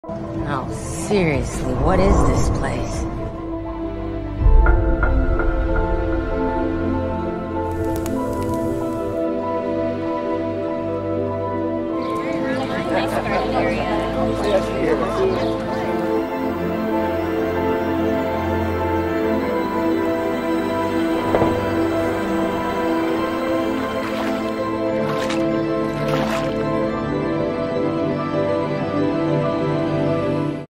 Living in a Dream | TeamLab Planets
A trip to an art museum in Tokyo, TeamLab Planets.
Shot with Sony A7IV, Sigma 24-70 f2.8 DG DN Art.
Graded w/ DaVinci Resolve.